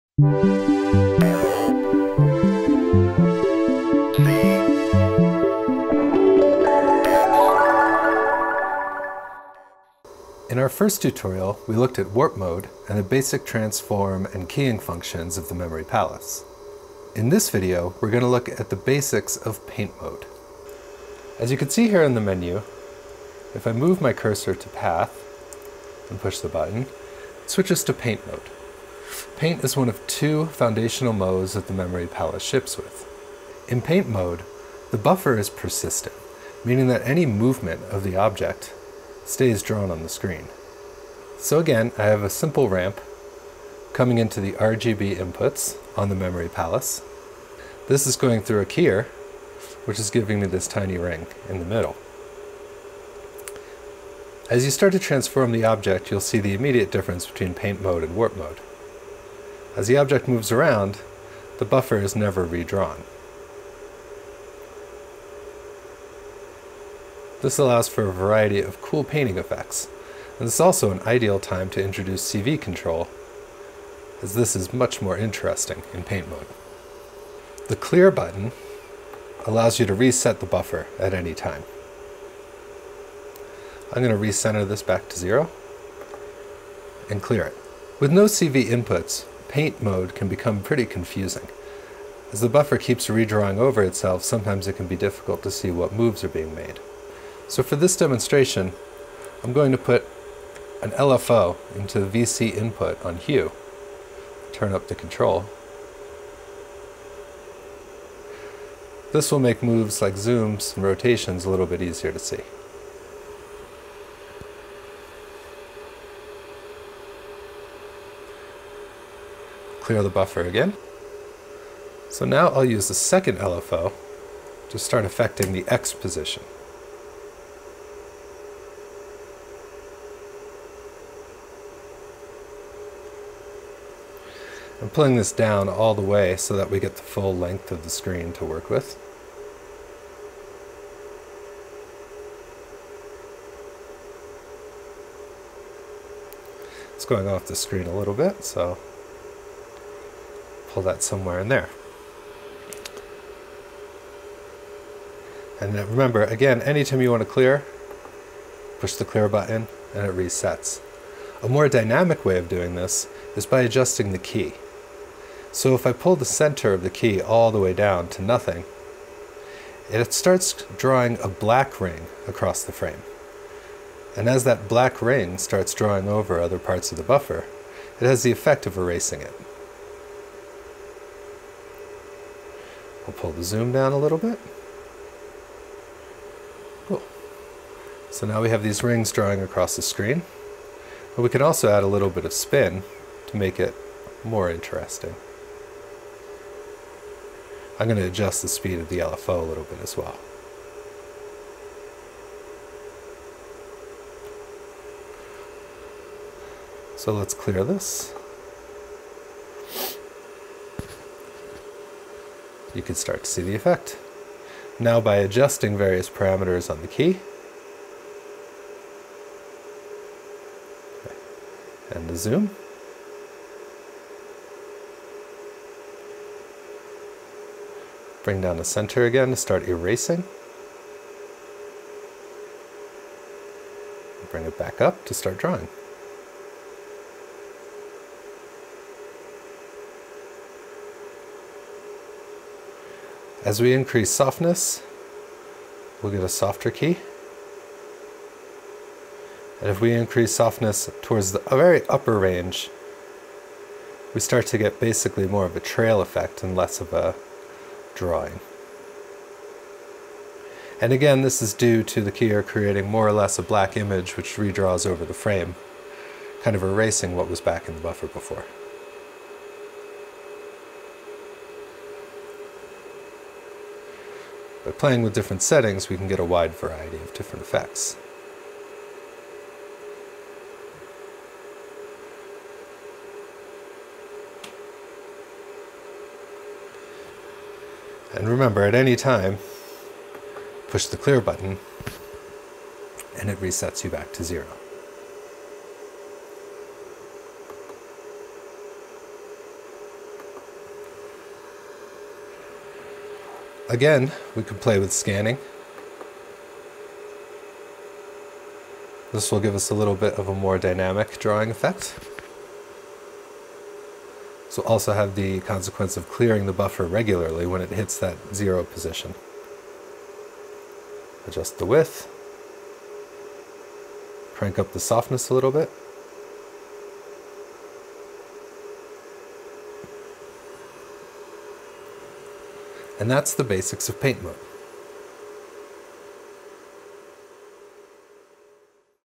LZX Memory Palace Tutorial 2: Paint Mode
LZX Memory Palace: 32-Bit ARGB Frame Buffer & Digital Video Effects module for EuroRack. presented by Johnny Woods.